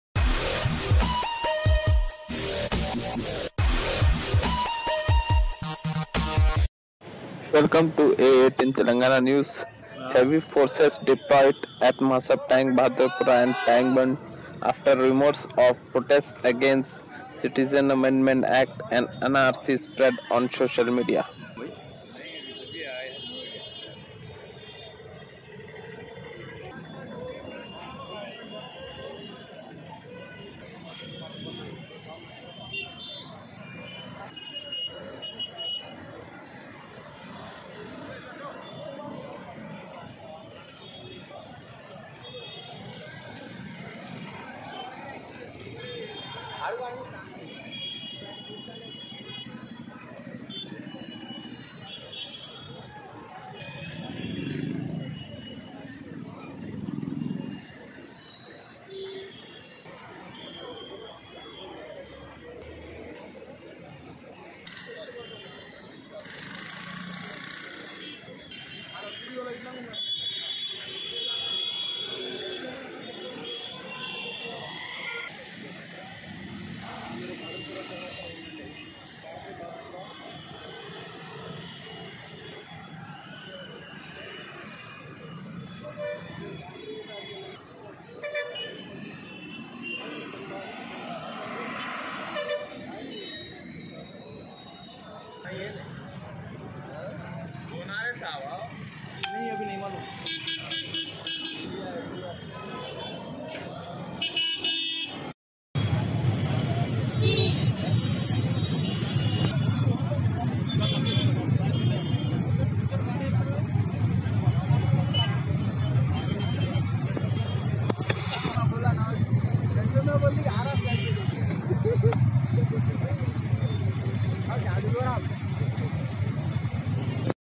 Heavy Forces Deployed at Masabtank, Bahadurpura and Tankbund After Rumours of Protest Against CAA an
Heavy Forces Deployed at Masabtank, Bahadurpura and Tankbund After Rumours of Protest Against CAA and NRC Spread on Social Media.